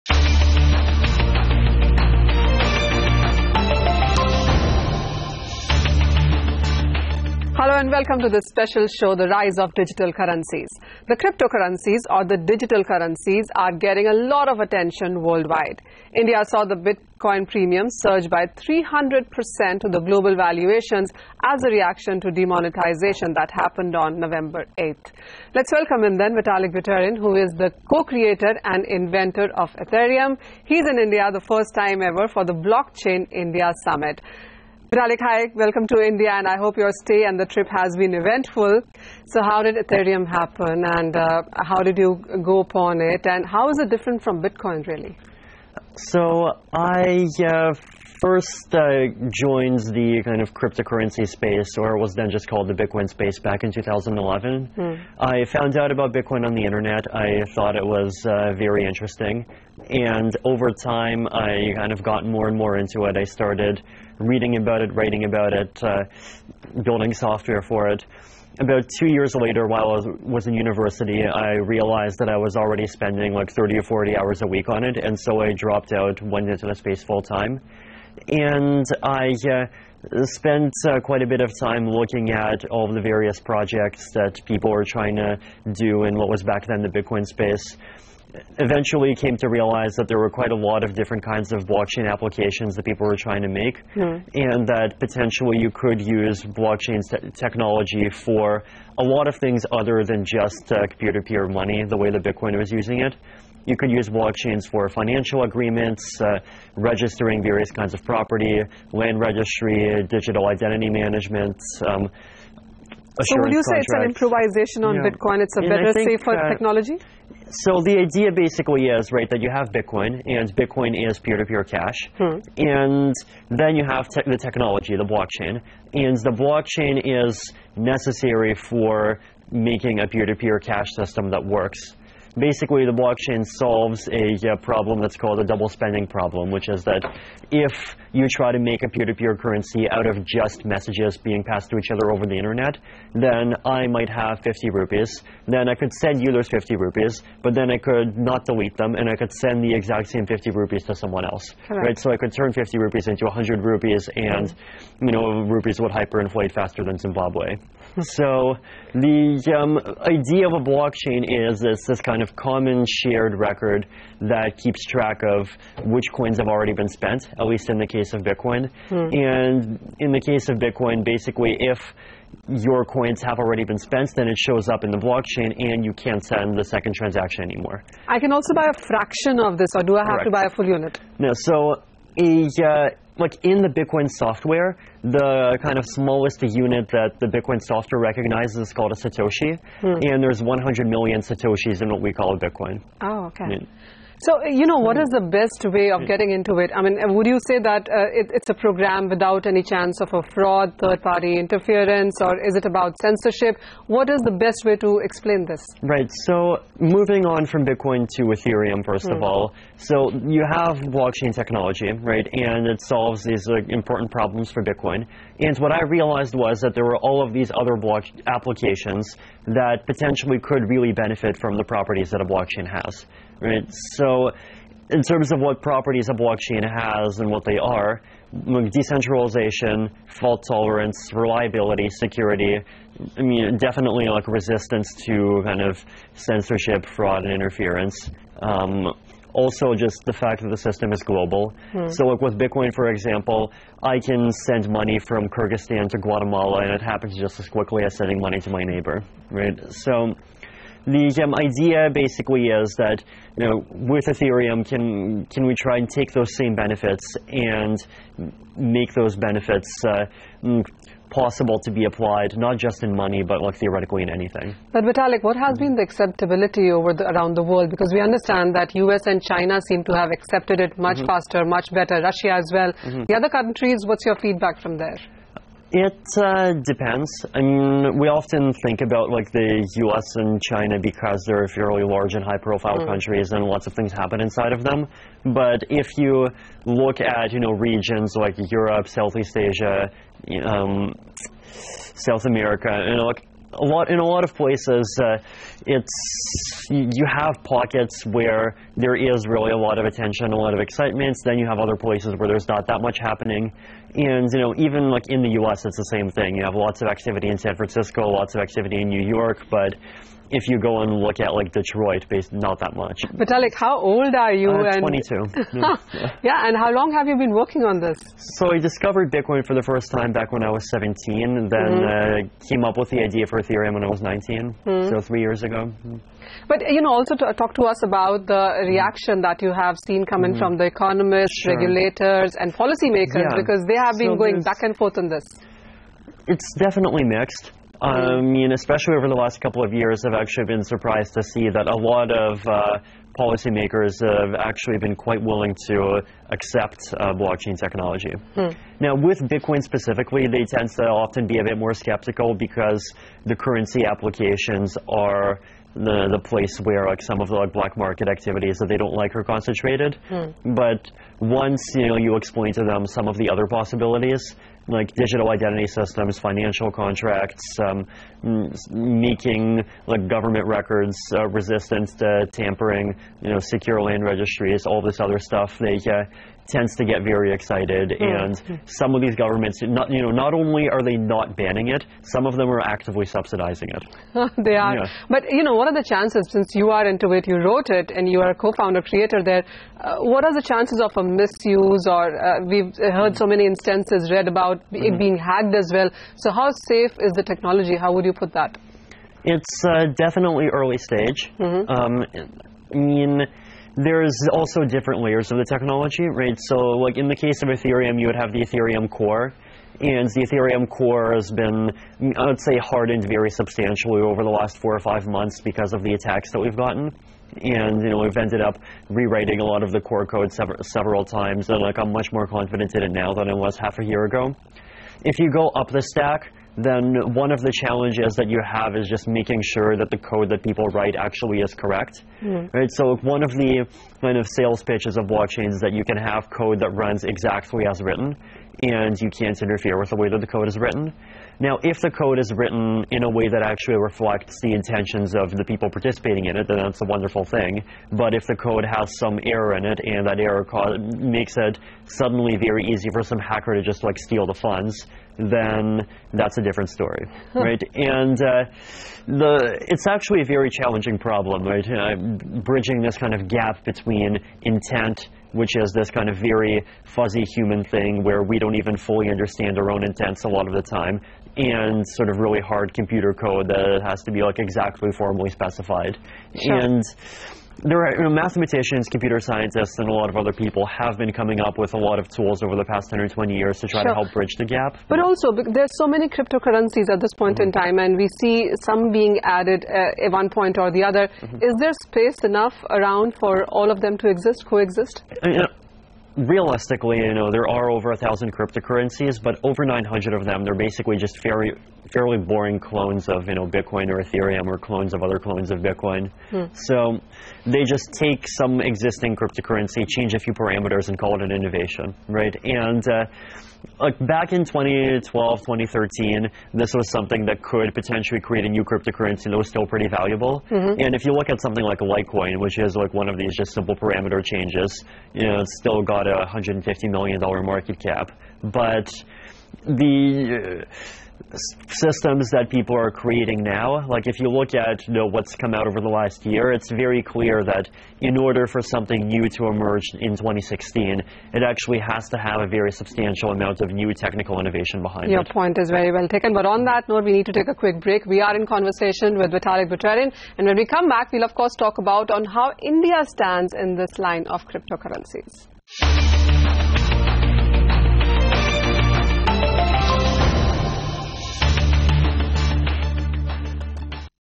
CNBC-TV18 Exclusive With Ethereum Creator (Part1) | Rise of Digital Currency
Vitalik Buterin, creator of the fastest growing currency speaks exclusively to CNBC-TV18's Manisha Gupta